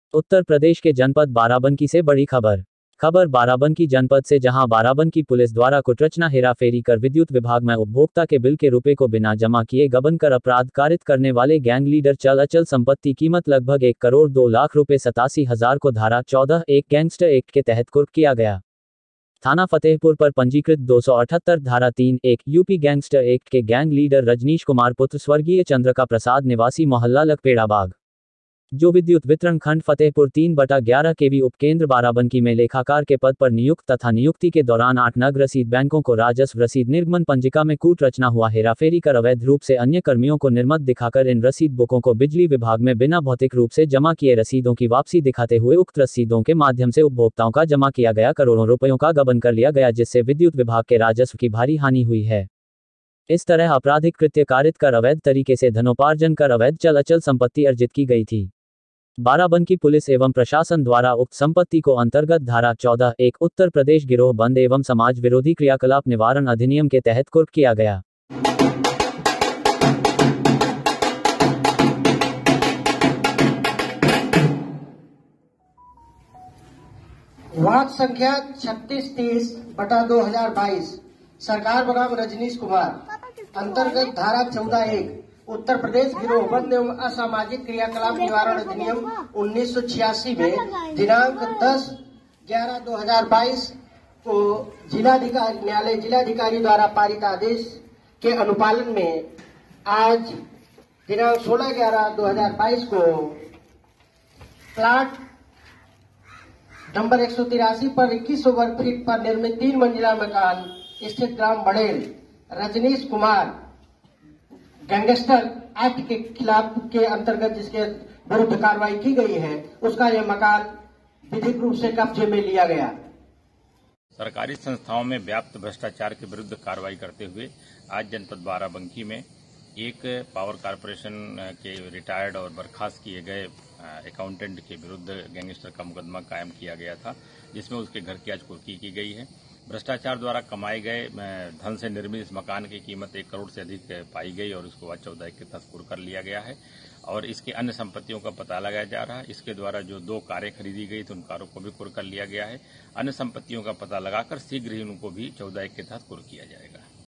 हेराफेरी कर विद्युत विभाग में बिल के रुपए को बिना जमा किए गबन करने वाले की चल अचल संपत्ति हुई कुर्क
उत्तर प्रदेश के जनपद बाराबंकी से बड़ी खबर

खबर बाराबंकी जनपद से जहां बाराबंकी पुलिस द्वारा कुटरचना हेरा फेरी कर विद्युत विभाग मैं उपभोक्ता के बिल के रुपए को बिना जमा किए गबन कर अपराध कारित करने वाले गैंग लीडर चल अचल संपत्ति कीमत लगभग 1 करोड़ 2 लाख ₹87000 को धारा 14 (1) गैंगस्टर एक्ट के तहत कुर्क किया गया

थाना फतेहपुर पर पंजीकृत 278/धारा 3(1) यूपी गैंगस्टर एक्ट के गैंग लीडर रजनीश कुमार पुत्र स्वर्गीय चंद्र का प्रसाद निवासी मोहल्ला लखपेड़ाबाग

जो विद्युत वितरण खंड फतेहपुर 3/11 केवी उप केंद्र बाराबंकी में लेखाकार के पद पर नियुक्त तथा नियुक्ति के दौरान 8 नग रसीद बैंकों को राजस्व रसीद निर्गमन पंजिका में कूट रचना हुआ हेराफेरी कर अवैध रूप से अन्य कर्मियों को निर्मत दिखाकर इन रसीद बुकों को बिजली विभाग में बिना भौतिक रूप से जमा किए रसीद ओं की वापसी दिखाते हुए उक्त रस्सी दों के माध्यम से उपभोक्ताओं का जमा किया गया करोड़ों रुपयों का गबन कर लिया गया जिससे विद्युत विभाग के राजस्व की भारी हानि हुई है

इस तरह अपराधिक कृत्य कारित कर अवैध तरीके से धनोपार्जन कर अवैध चल अचल संपत्ति अर्जित की गई थी

बाराबंकी पुलिस एवं प्रशासन द्वारा उक्त संपत्ति को अंतर्गत धारा 14 (1) उत्तर प्रदेश गिरोह बंद एवं समाज विरोधी क्रियाकलाप निवारण अधिनियम के तहत कुर्क किया गया